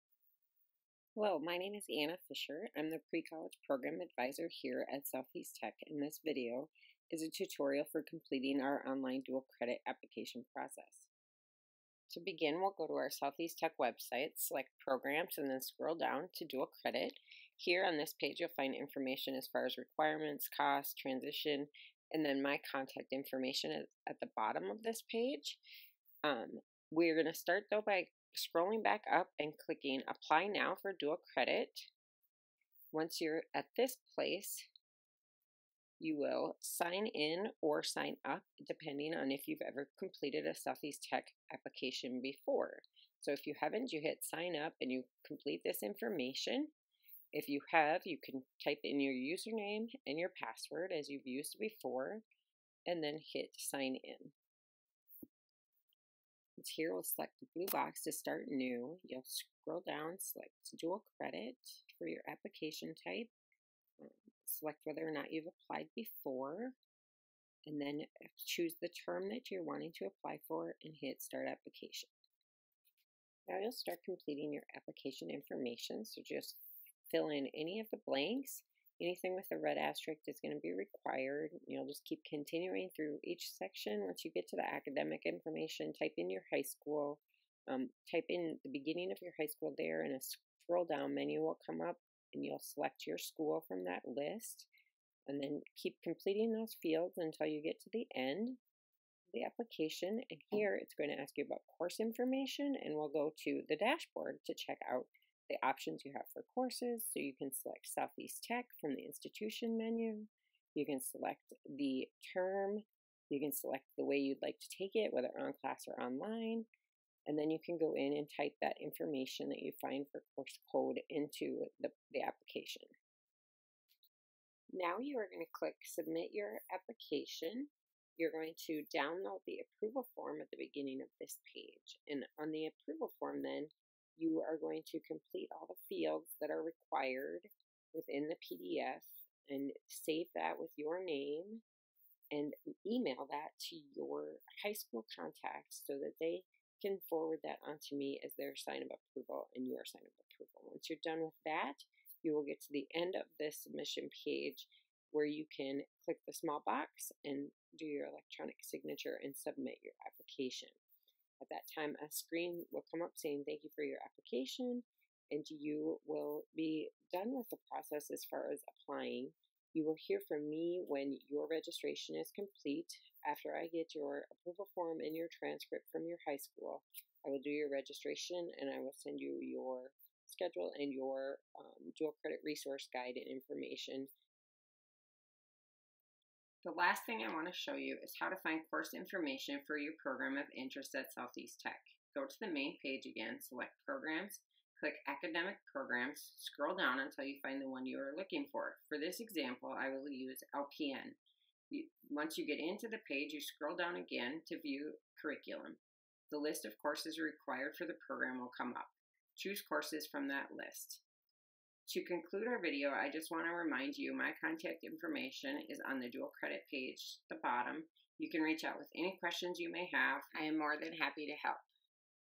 2020-21 Dual Credit Application Tutorial
Pre-College Program Advisor Anna Fischer walks high school applicants through the steps required to complete the application to enroll in Dual Credit Courses at  Southeast Tech in Sioux Falls, South Dakota.